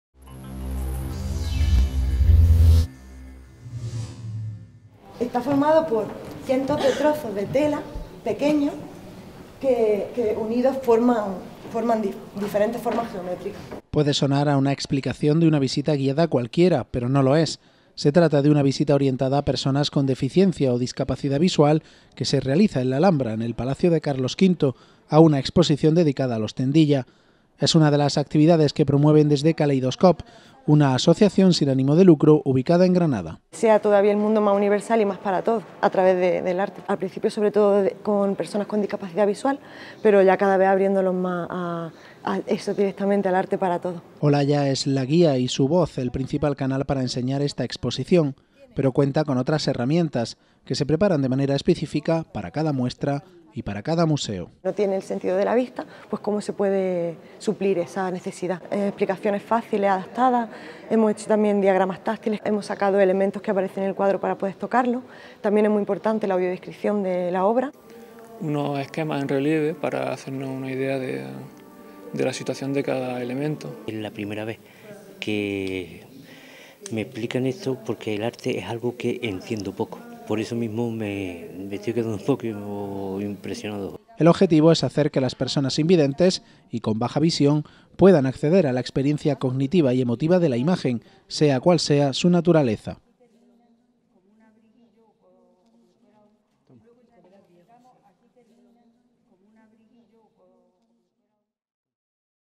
HDL Arte para todos con la voz y el tacto como guías para personas con discapacidad visual
Arte para todos. Es la premisa de Kaleidoscope, una asociación sin ánimo de lucro de Granada, que organiza y prepara visitas guiadas para personas con diferentes capacidades. A través de la audiodescripción y de material específico que preparan, acercan los contenidos de exposiciones y museos a personas con discapacidad visual.